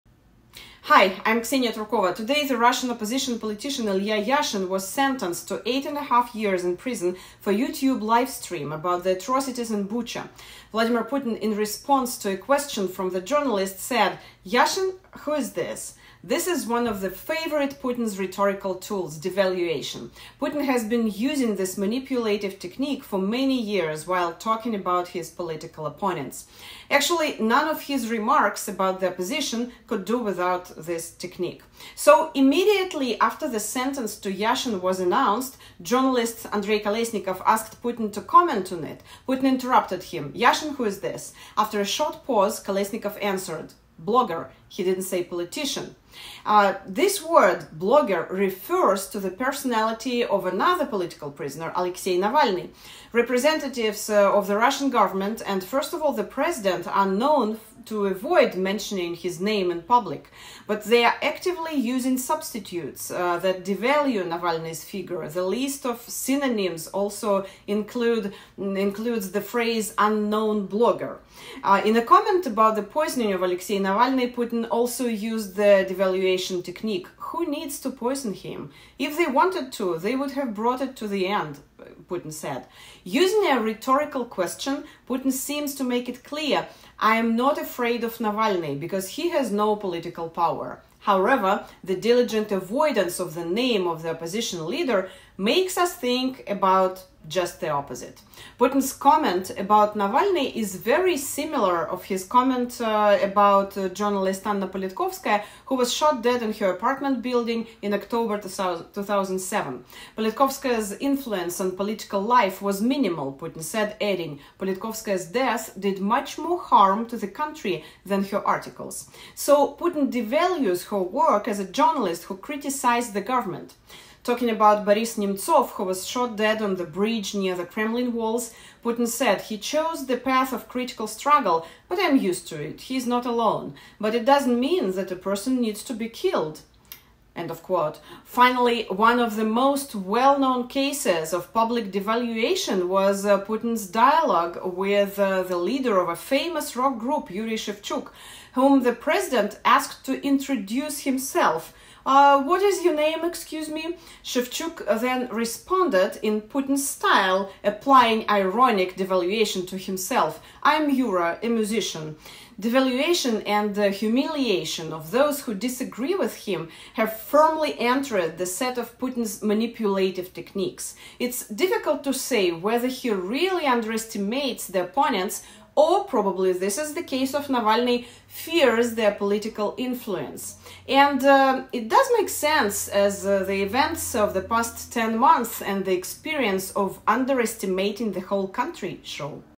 “Yashin? Who is that?” Putin’s tactics of humiliation and devaluation
Russian opposition politician Ilya Yashin was sentenced to eight and a half years in prison for YouTube live stream about the atrocities in Bucha. Vladimir Putin, in response to a question from the journalists, said: “Yashin? Who is this?"

This is one of favorite Putin's rhetorical tools — devaluation. Putin has used this manipulative technique for years while talking about his political opponents.